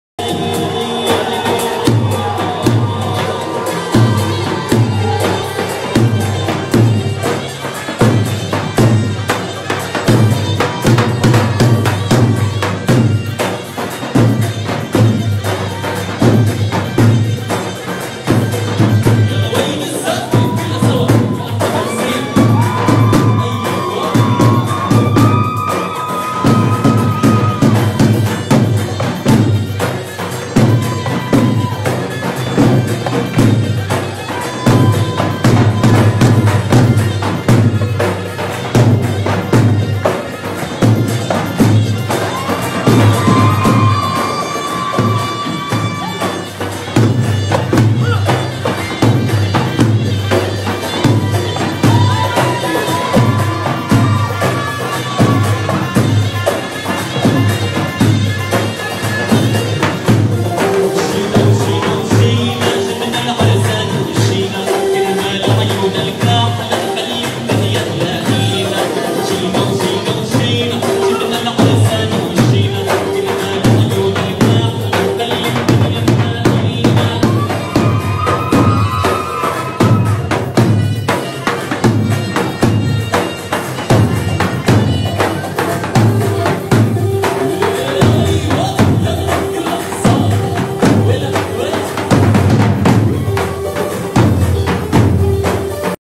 Adelaide Best wedding Drummers
Raw footage from the band presenting at a client's wedding, such an enormous amount of energy and hype.